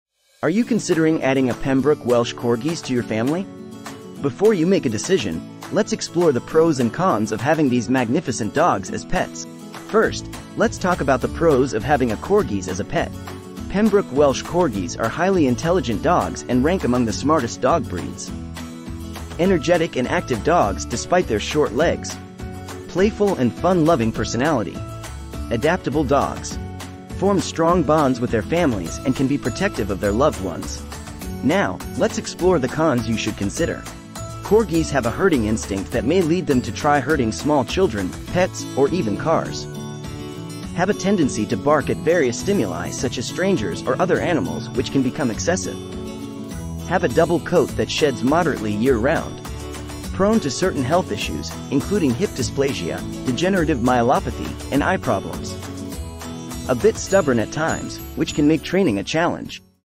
DWARF DOG! Things You Need To Know Before Buying A Pembroke Welsh Corgi - PROS & CONS
Are you considering adding a Pembroke Welsh Corgi to your family? This video shows the things you need to know before buying or having a Corgi dog as a pet. You will know the pros and cons and let you decide if it's the ideal dog breed you are looking for.